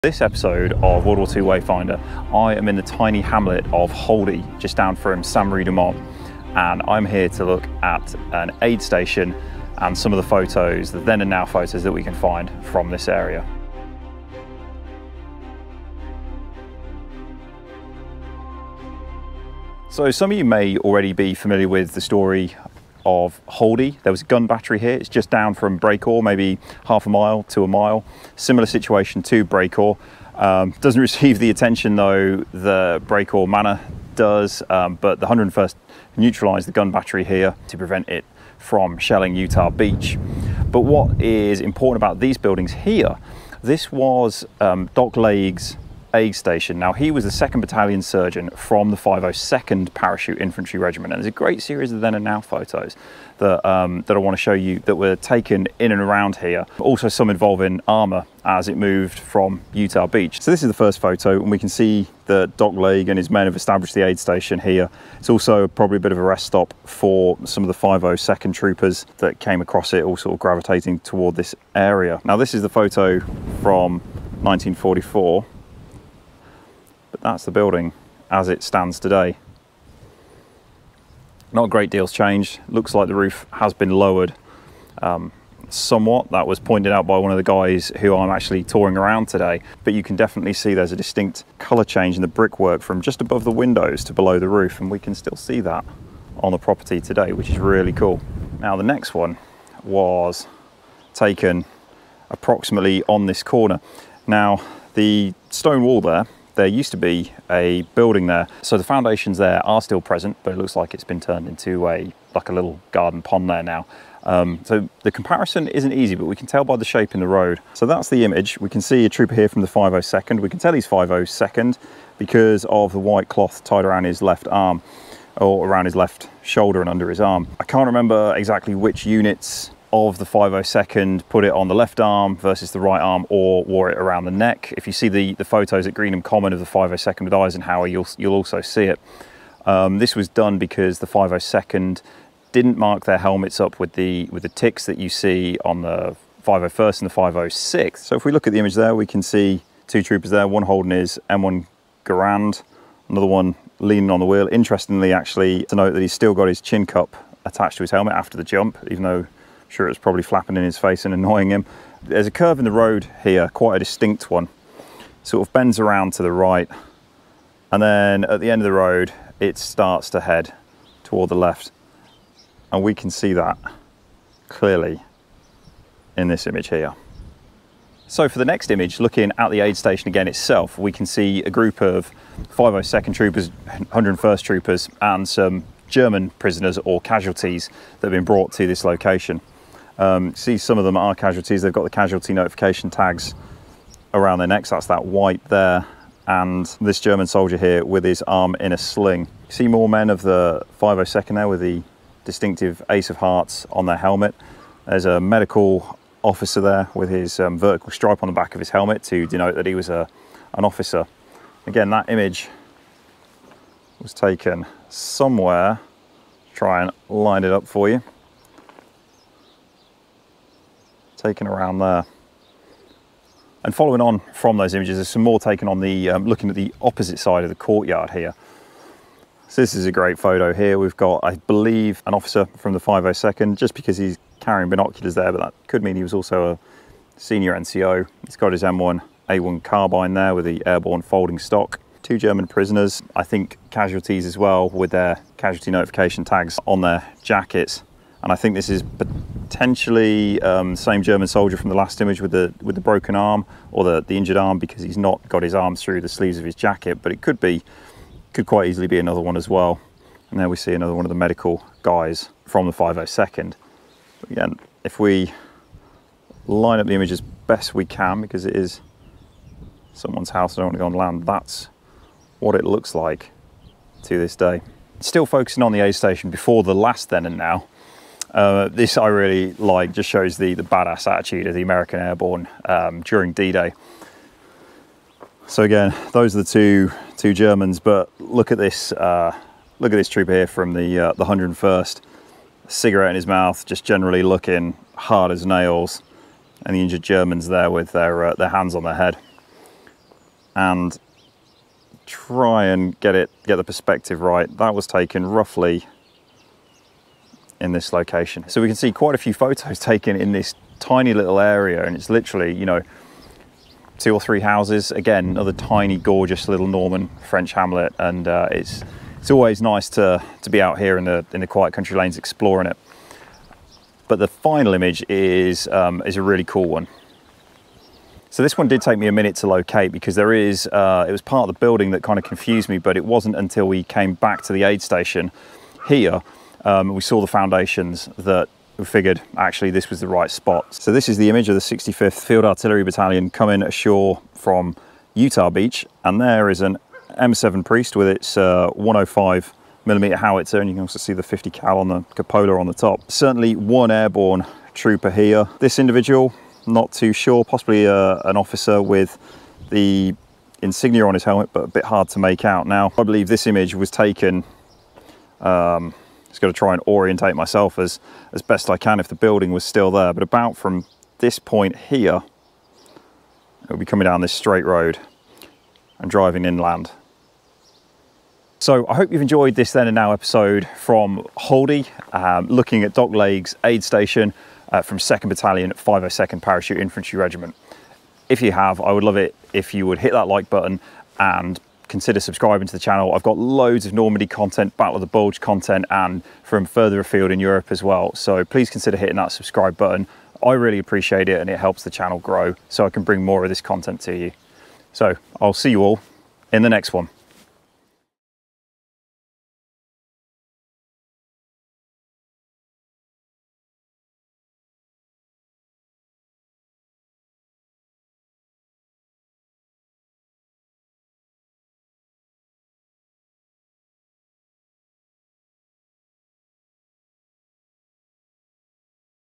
Airborne Medics on D-Day - 101st Aid Station Then and Now
The medics on D-Day did an exceptional job, but their role is often overlooked.

For this WW2 Wayfinder episode I look at the Aid Station of Doc Lage, the 2nd Battalion Surgeon for and his Medics from the 502nd PIR, 101st airborne at Holdy near the village of Sainte Marie Du Mont in Normandy on D-Day, 6th June 1944.